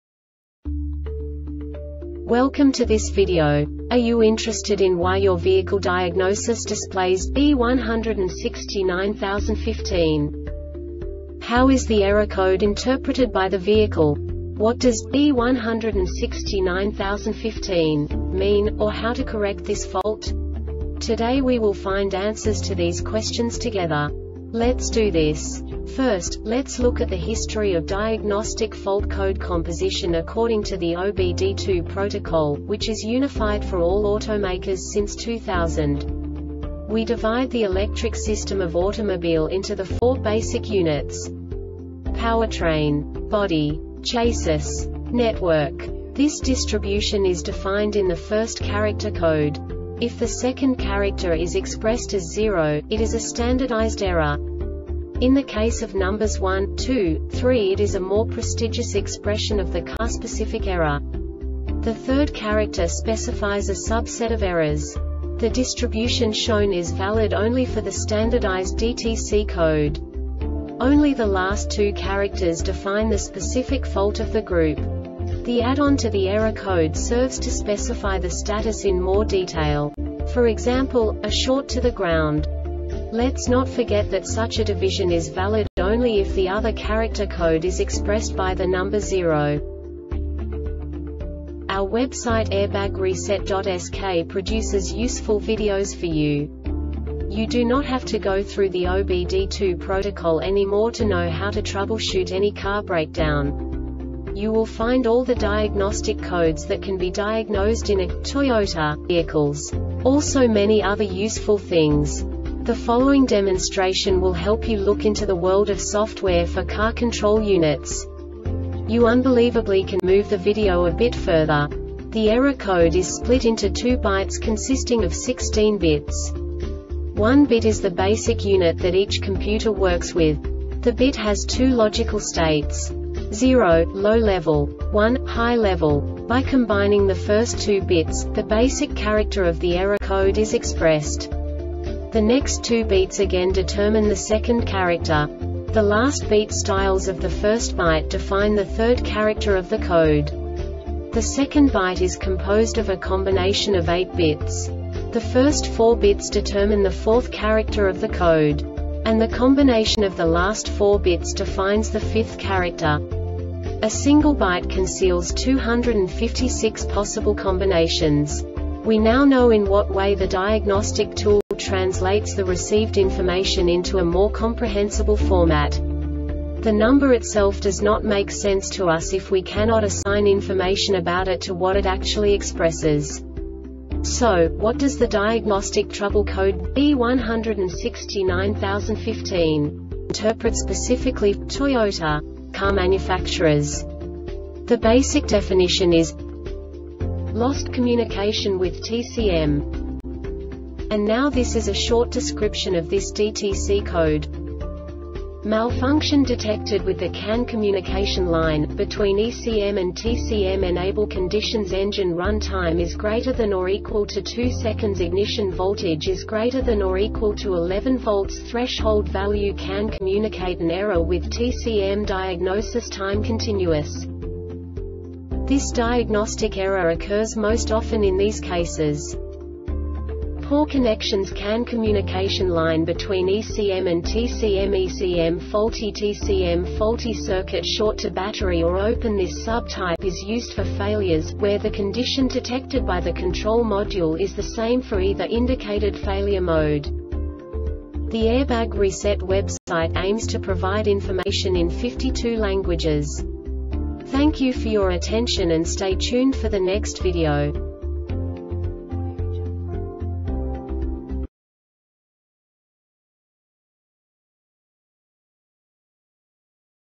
DTC Toyota B1690-15 Short Explanation
Contents:
0:21 Basic DTC analysis according to OBD2 protocol standard.
1:48 Insight into programming
2:58 A diagnostically understandable description of the Diagnostic Trouble Code
3:05 Basic error definition

Make:
Toyota
Code:
B1690 15
Definition:
Lost Communication With TCM
Description: